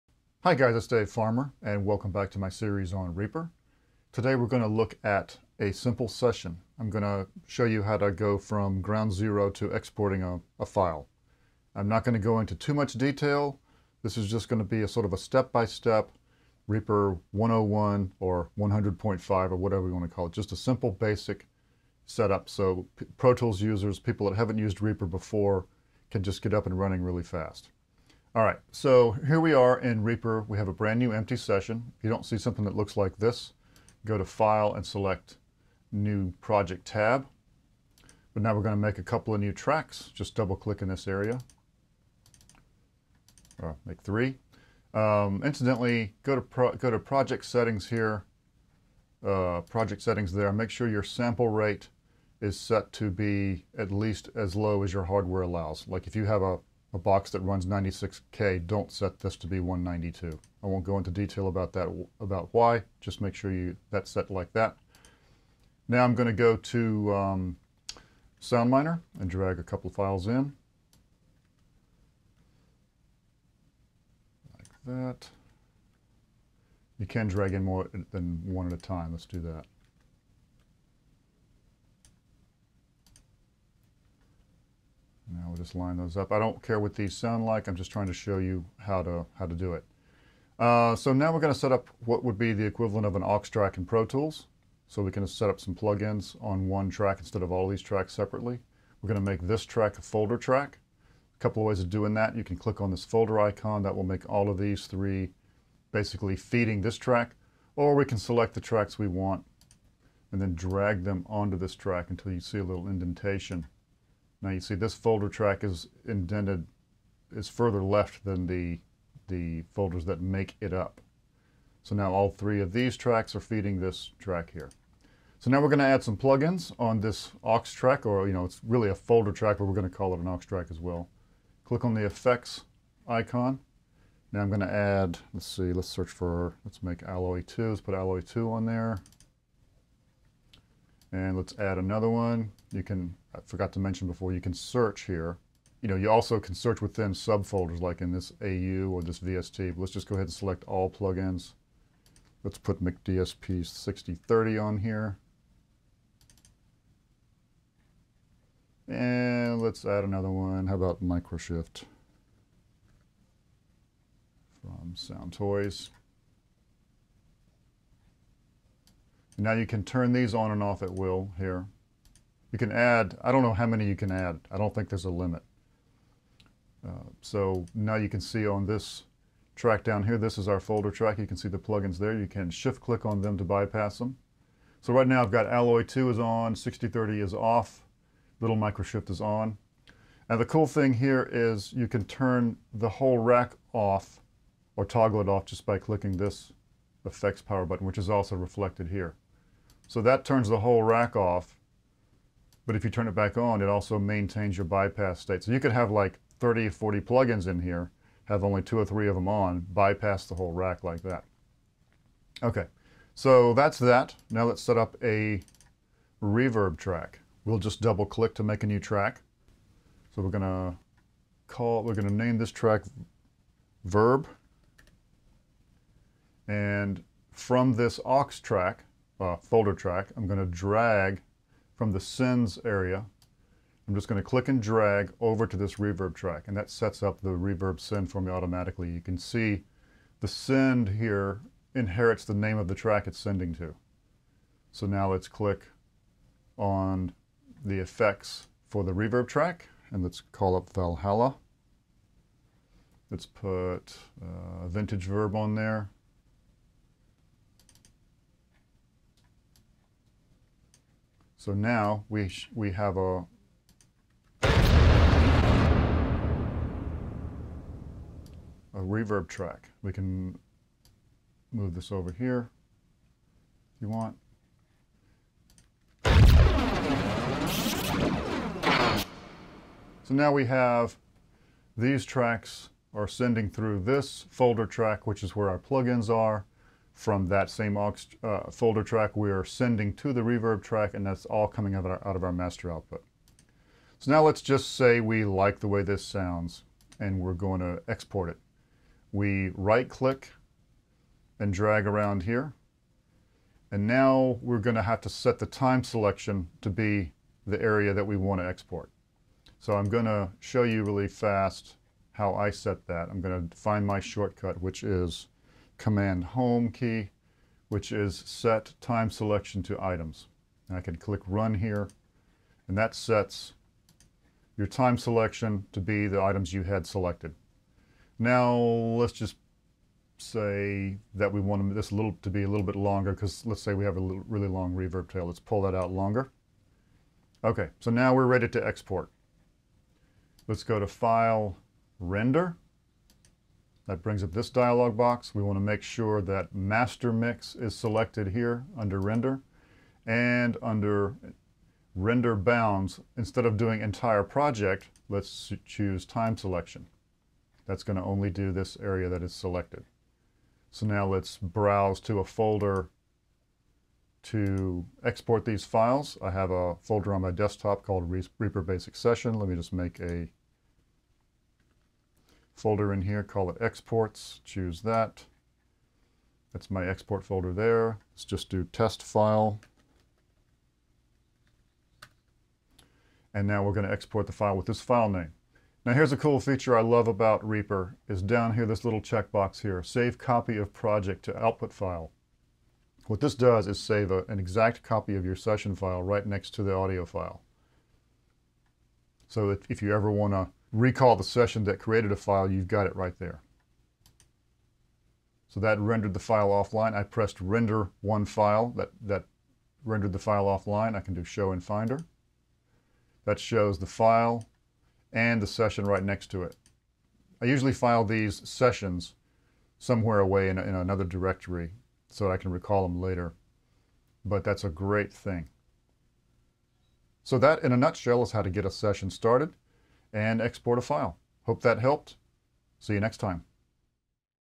Reaper 006 - Basic Session startup to exporting a file - Beginners Tutorial
This quick tutorial shows new users how to go from an empty session, to adding some plugins, set up a send to a reverb "aux", and export a file.